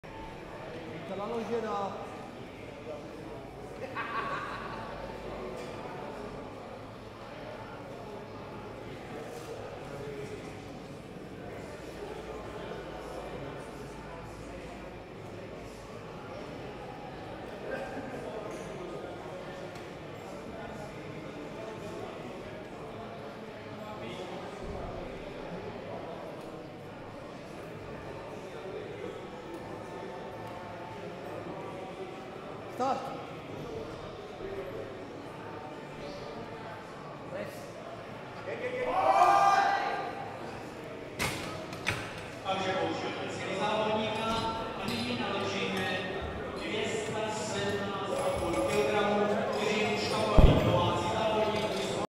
Pšenica Radek (1991), BP3, 165 kg (x)
4. Mistrovství Moravy v silovém trojboji družstev

Místo: Sportovní hala, Krnov